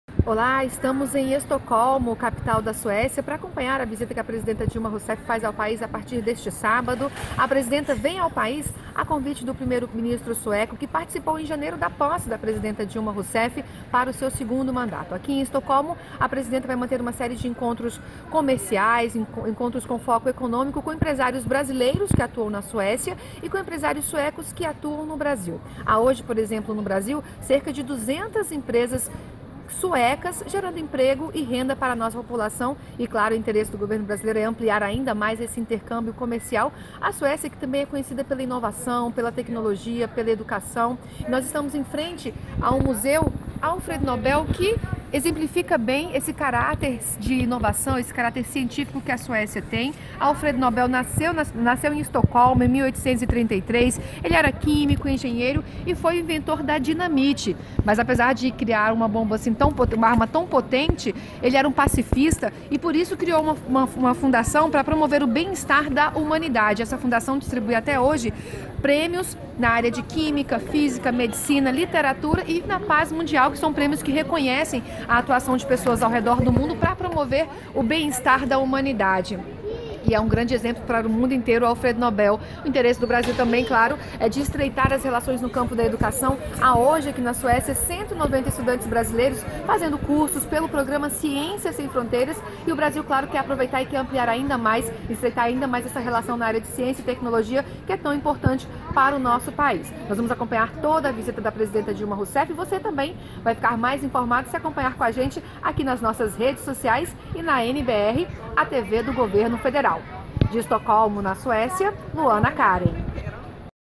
Equipe da TV NBR está na Suécia para cobertura de visita oficial de Dilma Rousseff
ESPECIAL WEB - 16.10.15: A presidenta Dilma Rousseff viaja neste sábado (17) para a Suécia, onde realiza visita de estado. A equipe da TV NBR já está em Estocolmo, capital do país escandinavo, para realizar a cobertura. Dilma terá uma série de encontros com objetivo de ampliar laços comerciais, tecnológicos e educacionais entre os dois países.